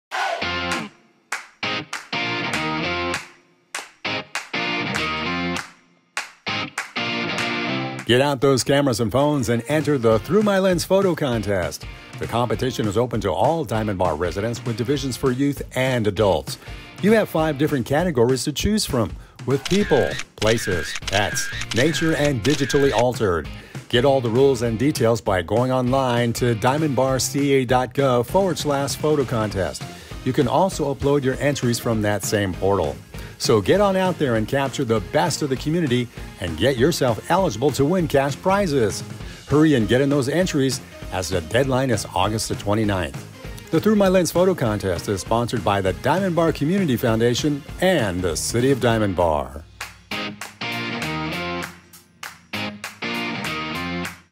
2022 Through My Lens Photo Contest
The Diamond Bar Through My Lens Photo Contest is open for your entries. The contest is open to all Diamond Bar residents as we have divisions for Youth and Adults. Its free and easy to enter: